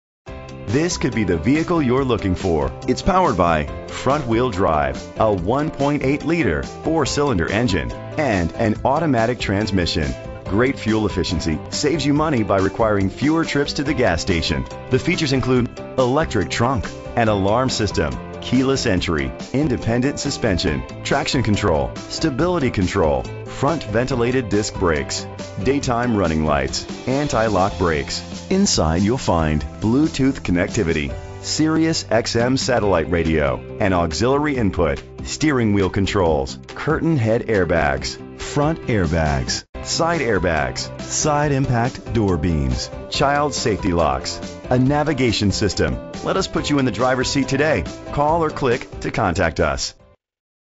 2016 Chevrolet Cruze Limited 16510DT - Homewood IL
Year: 2016
Make: Chevrolet
Model: Cruze Limited
Trim:
Engine: 1.8 liter 4 Cylindercylinder FWD
Transmission: Automatic
Color: Black
Mileage: 2
Address:
18033 Halsted St
Homewood, IL 60430
VIN:1G1PC5SH4G7212349